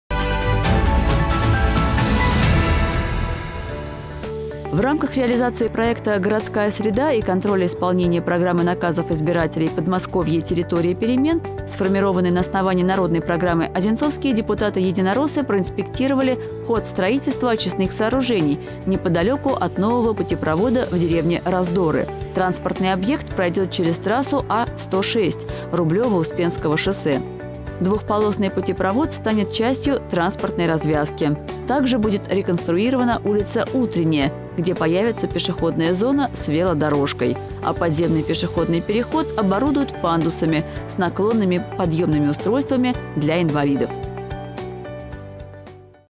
Очистные сооружения в Раздорах
Депутаты-единороссы проверили процесс возведения очистных сооружений в Раздорах. Их строят неподалеку от нового путепровода.